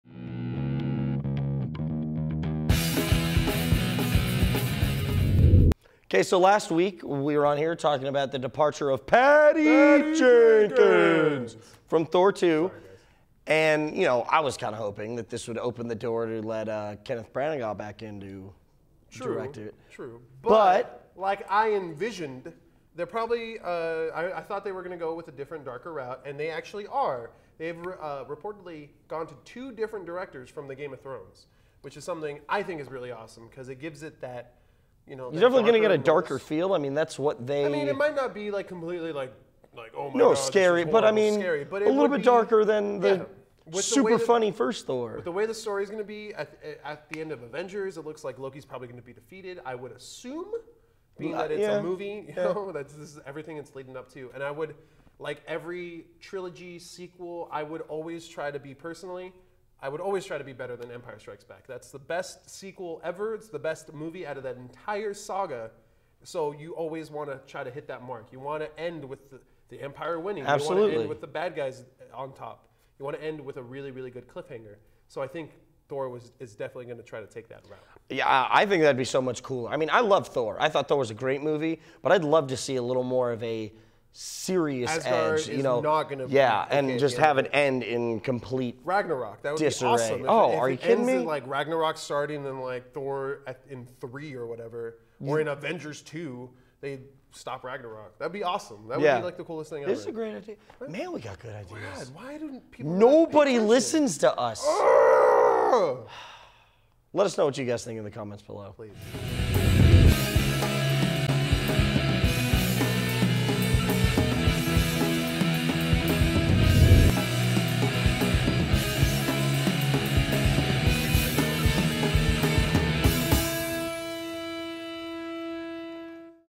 Nerdlocker - Thor 2 Updates!
- Cubby and Brandon talk more Thor 2 news. For more info on comics, video games, movies and anything else nerd, check out Nerdlocker.com, a place for your inner nerd.